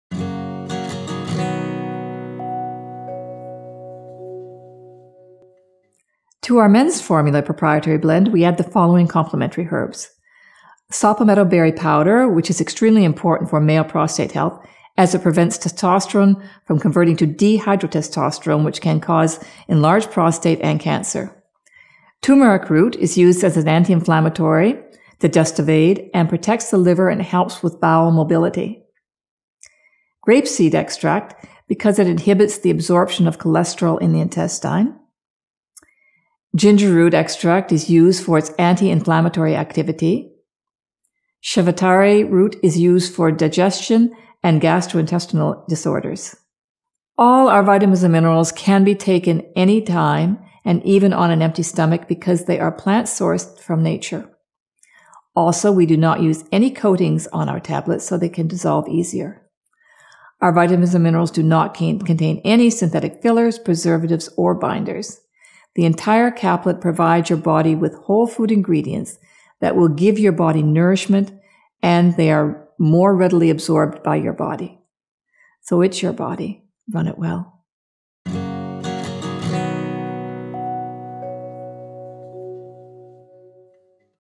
Nova Scotia Organics - Men's Multivitamins & Minerals
Learn more about our Multivitamins & Minerals made primarily from plant sourced vitamins and minerals with added herbs which complement each formula. Nova Scotia Organics manufactures four different multivitamins: Men's Multivitamins & Minerals, Women's Multivitamins & Minerals, Women's 50+ Multivitamins & Minerals and Pre & Post Natal Multivitamins & Minerals.